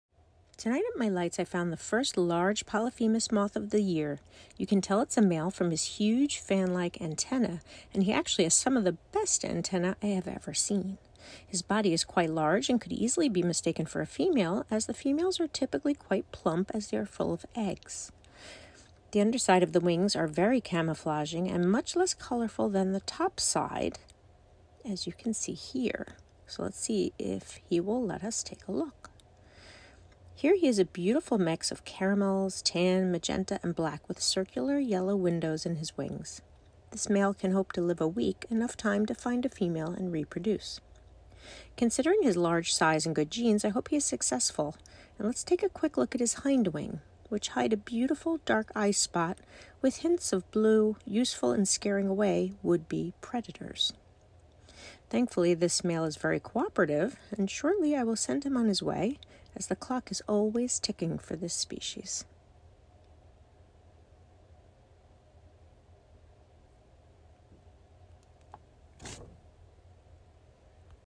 Male Polyphemus Moth
Common in North America, the Polyphemus moth is one of the most endearing and common species, widely bred by enthusiasts.